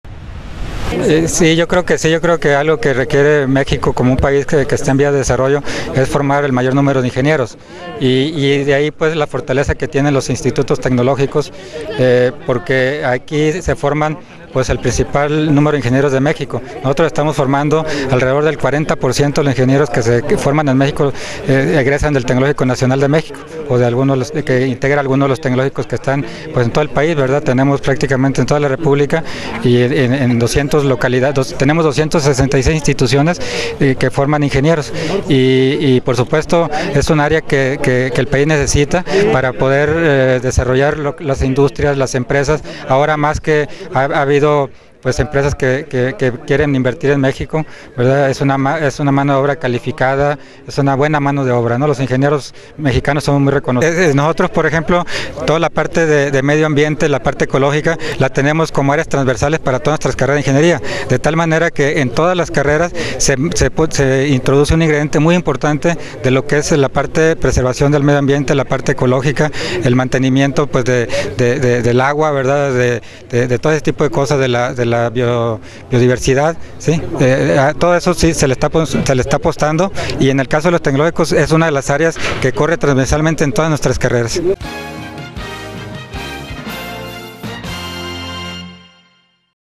Ingenierías ganan terreno sobre carreras humanistas
Los egresados de las ingenierías que se ofrecen en los Institutos Tecnológicos, tienden a colocarse rápidamente en los espacios laborales, afirmó Manuel Quintero Quintero, Director General de los Institutos Tecnológicos. Agregó que actualmente la tendencia de crecimiento del país, por estar en vías de desarrollo es la demanda de ingenieros o técnicos, de tal manera que hoy son más los jóvenes que se inclinan por una carrera de este tipo, revirtiéndose así la histórica tendencia al estudio de carreras humanistas.